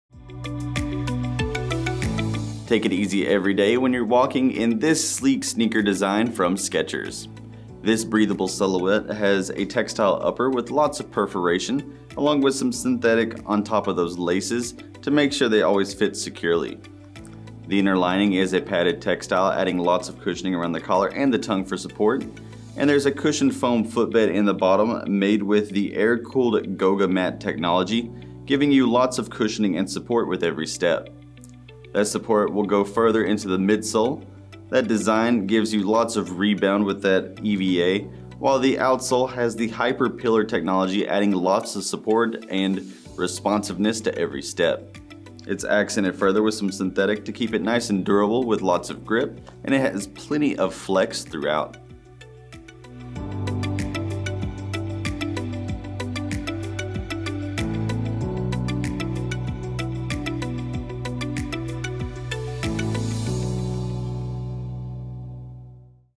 SKECHERS Performance Go Walk 6 - 216209 SKU: 9586111
Boasting a sleek design, the  SKECHERS&reg; Go Walk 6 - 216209 shoes provide optimal comfort to your feet.
Mesh upper.
Breathable textile lining.
Cushioned insole ensures maximum comfort.
Brand name embossed on the tongue and the heel tab.
High-rebound ultra-lightweight Hyper Pillar Technology&trade; for added support.
Classic round toe.
Pull tabs for easy on/off.
Lace-up closure.
Rubber traction outsole for added stability.
Imported.